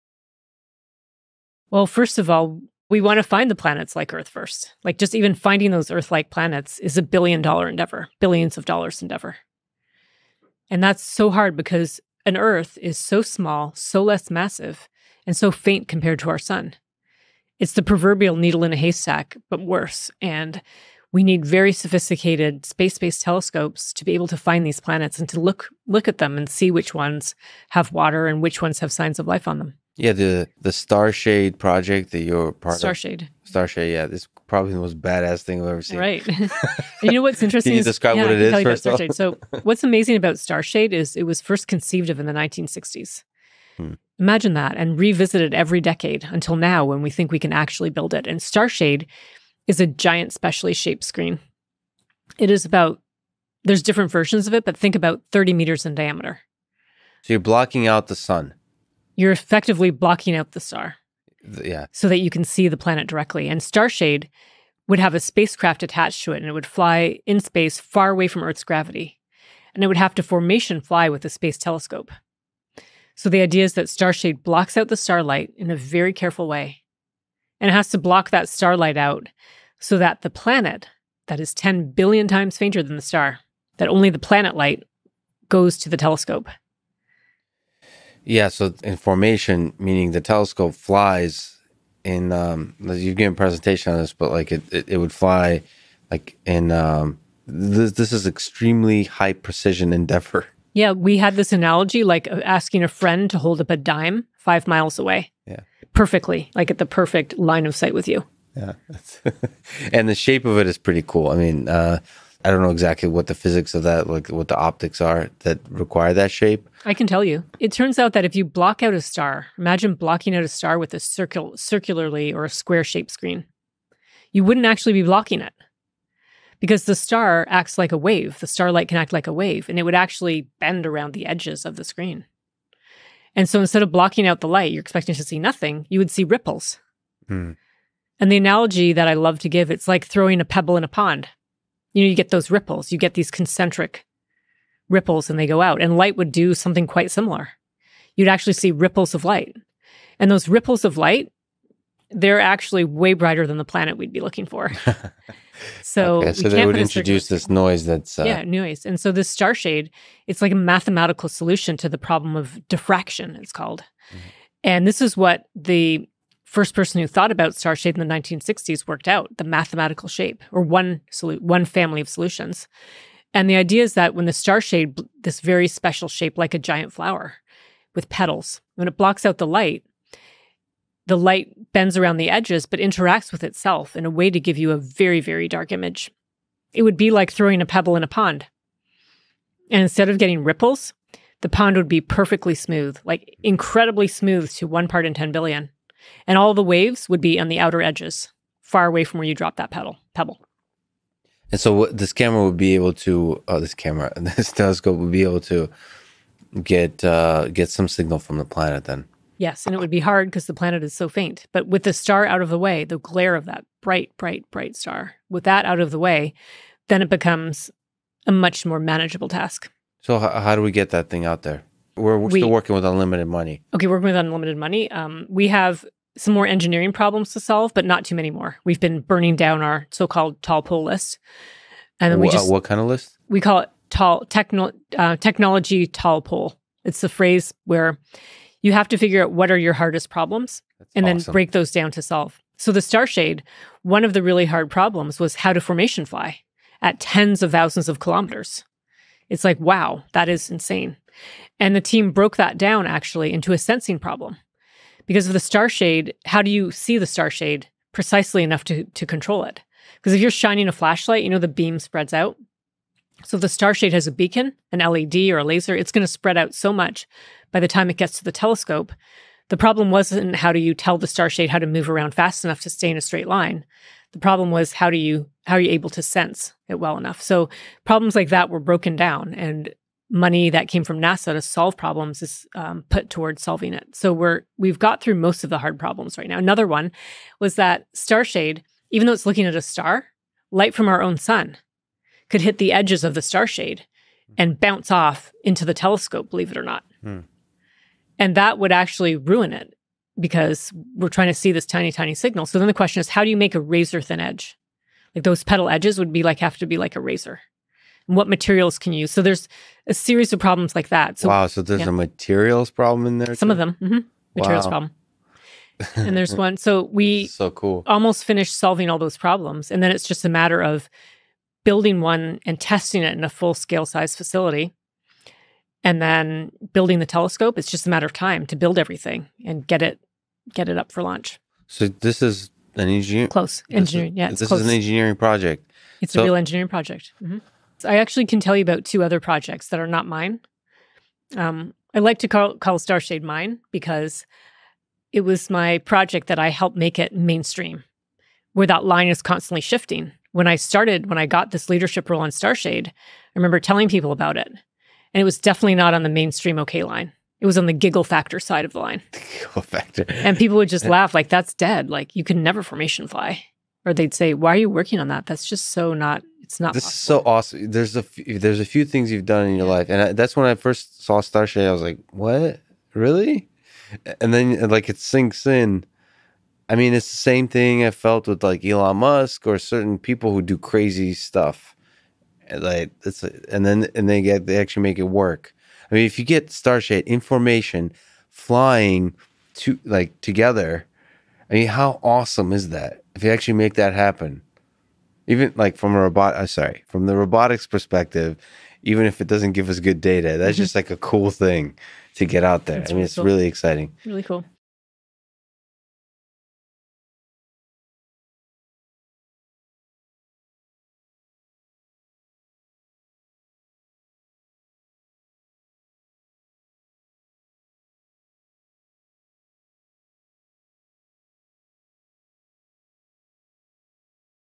Starshade: Flower-shaped telescope screen for taking pictures of earth-like planets | Sara Seager
Sara Seager is a planetary scientist at MIT, known for her work on the search for exoplanets.